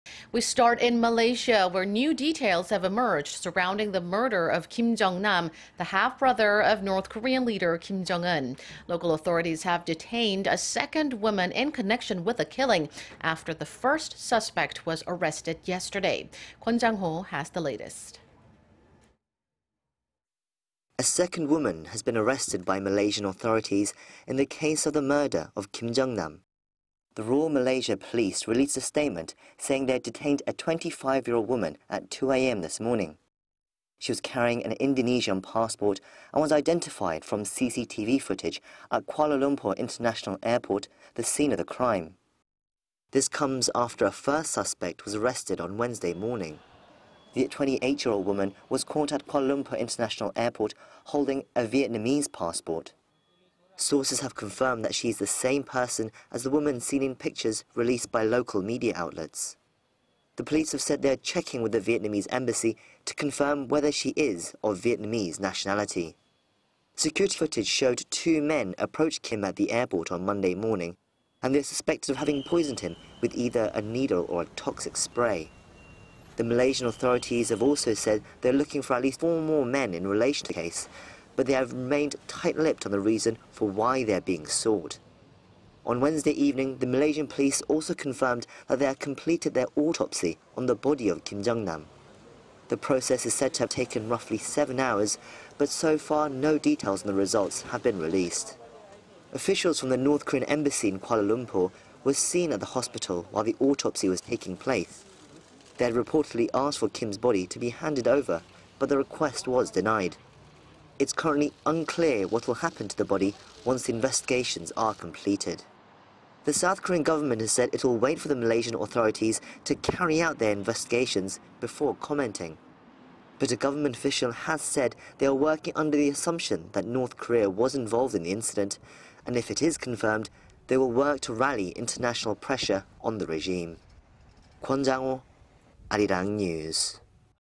Second woman arrested in Kim Jong-nam murder case
"말레이 경찰, 김정남 살해 여성 용의자 1명 추가 체포"
We start in Malaysia,... where new details have emerged surrounding the murder of Kim Jong-nam, the half brother of North Korean leader Kim Jong-un.
Local authorities have detained a second woman in connection with the killing... after the first suspect was arrested yesterday.
Kwon Jang-ho has the latest.
A second woman has been arrested by Malaysian authorities in the case of the murder of Kim Jong-nam.
The Royal Malaysia Police released a statement, saying they had detained a 25 year old woman, at 2 a.m. this morning.
She was carrying an Indonesian passport and was identified from CCTV footage at Kuala Lumpur International Airport, the scene of the crime.

This comes after a first suspect was arrested on Wednesday morning.
The 28-year-old woman was caught at Kuala Lumpur International Airport... holding a Vietnamese passport.
Sources have confirmed that she is the same person as the woman seen in pictures released by local media outlets.
The police have said they are Zchecking with the Vietnamese embassy to confirm whether she is of Vietnamese nationality.

Security footage showed two women approaching Kim at the airport on Monday morning, and they are suspected of having poisoned him with either a needle or a toxic spray.
The Malaysian authorities have also said they are looking for at least four more men in relation to the case, but they have remained tight-lipped on the reason for why they are being sought.

On Wednesday evening, the Malaysian police also confirmed that they had completed their autopsy on the body of Kim Jong-nam.
The process is said to have taken roughly seven hours, but so far no details on the results have been released.
Officials from the North Korean embassy in Kuala Lumpur were seen at the hospital while the autopsy was taking place.
They had reportedly asked for Kim's body to be handed over, but the request was denied.
It's currently unclear what will happen to the body once the investigations are completed.

The South Korean government has said it will wait for the Malaysian authorities to carry out their investigations before commenting.
But a government official has said they are working under the assumption that North Korea was involved in the incident and if it is confirmed they will work to rally international pressure on the regime.